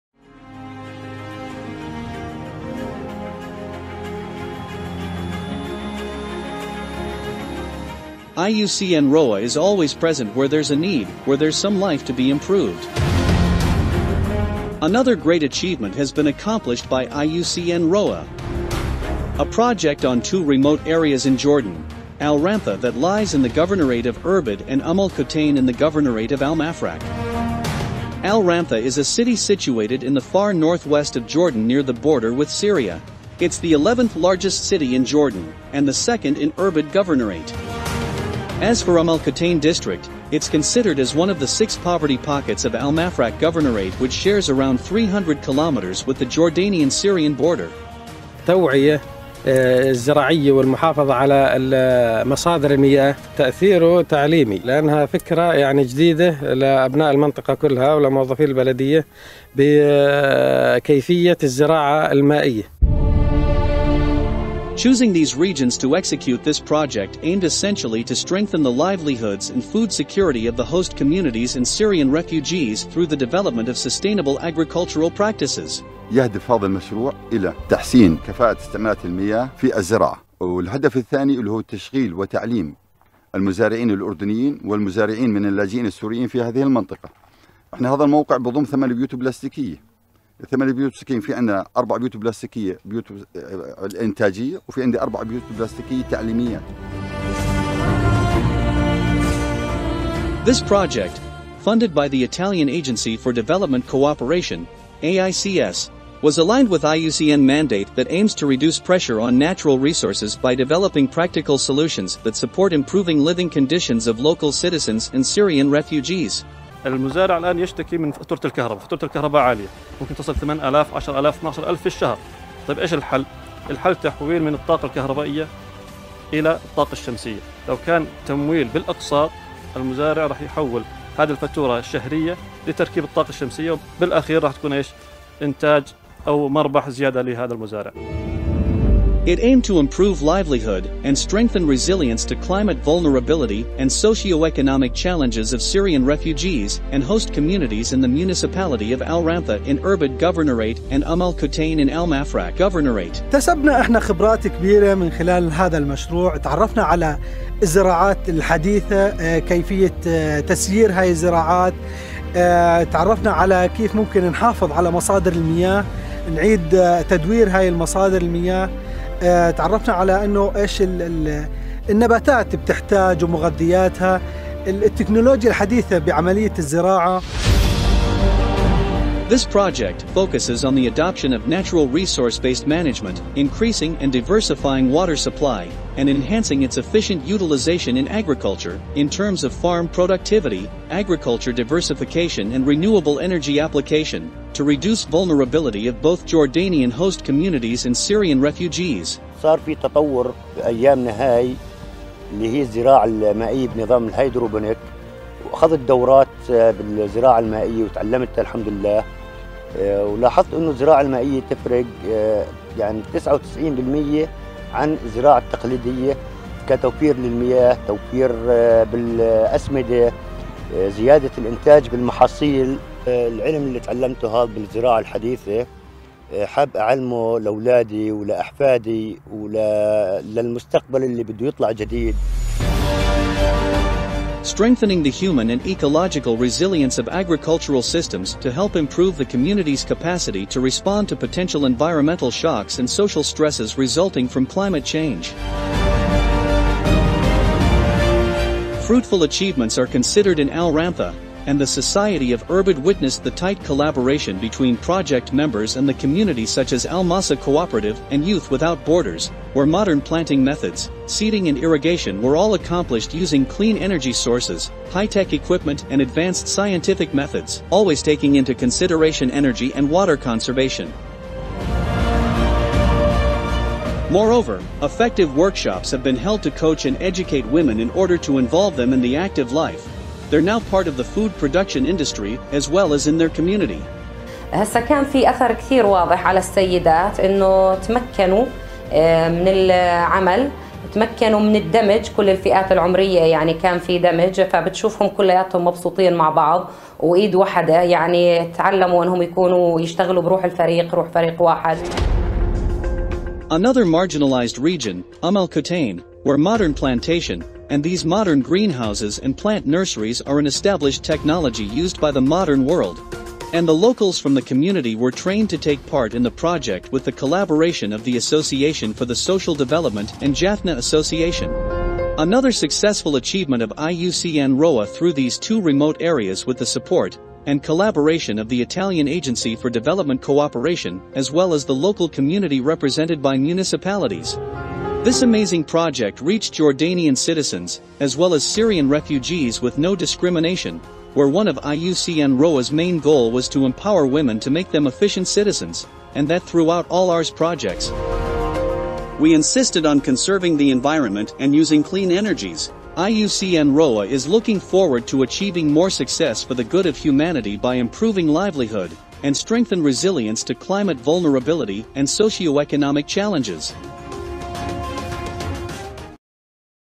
IUCN ROWA Water and Climate Change Programme Project
Strengthening the Livelihoods and Food Security of the Host Communities and Syrian Refugees through the Development of Sustainable Agricultural Practices project is aligned with IUCN mandate through the contributing improvement of the living conditions of the Jordanian and Syrian refugees, and development of practical solutions to reduce the pressure on natural resources.
The project aims to improve livelihood, and strengthen resilience to climate vulnerability and Socio-Economic challenges of Syrian refugees, and host communities in the municipality of Ramtha (Irbid, Governorate) and Um Al-Qaten (Mafraq, Governorate).

The Project is considered as a model of the NEXUS approach, to address the unique sustainability challenges and opportunities that face the local level of municipalities regarding water, energy, and food security. In order to achieve the project’s goals.